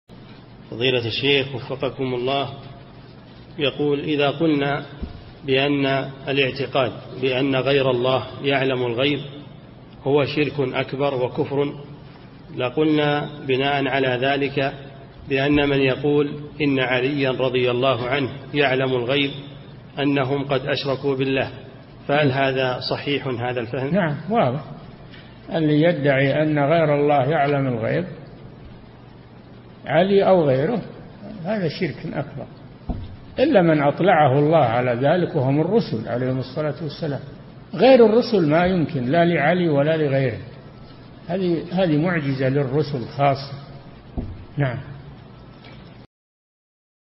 [265 -939] هل الذين قالوا إن عليا رضي الله عنه يعلم الغيب قد أشركوا بالله؟ - الشيخ صالح الفوزان
-[265 -939] السؤال: إذا قلنا بأن الاعتقاد بأن غير الله يعلم الغيب هو شرك أكبر وكفر لقلنا بناء على ذلك بأن من يقول إن علي رضي الله عنه يعلم الغيب أنهم قد أشركوا بالله، فهل هذا صحيح الفهم؟
الجواب: نعم واضح، الذي يدعي أن غير الله يعلم الغيب، علي أو غيره فهذا شرك أكبر، إلا من أطلعه الله على ذلك وهم الرسل عليهم الصلاة والسلام، غير الرسول ما يمكن لا لعلي ولا لغيره، هذه معجزة للرسل خاصة.
للاشتراك في قناة الشيخ صالح الفوزان

للاستفسار والإقتراحات والدعم
رئيس الفريق
لمشروع كبار العلماء